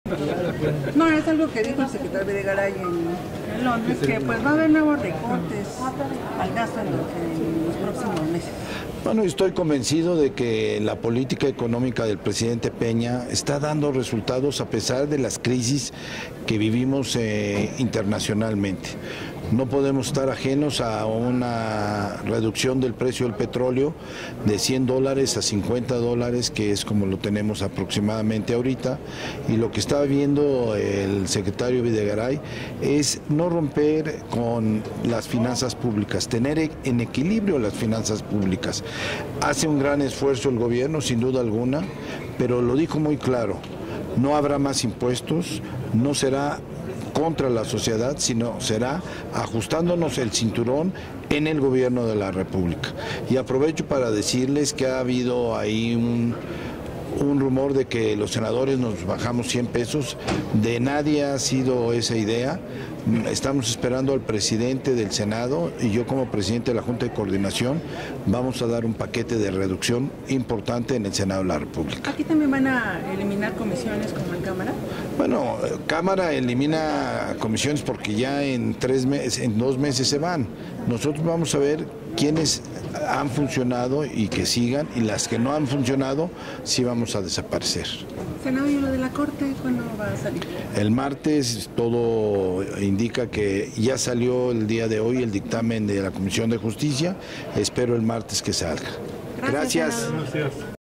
La política económica del Gobierno Federal da resultados a pesar de las crisis: Senador Gamboa
Entrevista al Coordinador del PRI en el Senado de la República, Emilio Gamboa Patrón.
Desmintió las versiones de que los senadores se hayan reducido 100 pesos de dieta. Aseguró que habrá reducciones en las actividades del Senado de la República.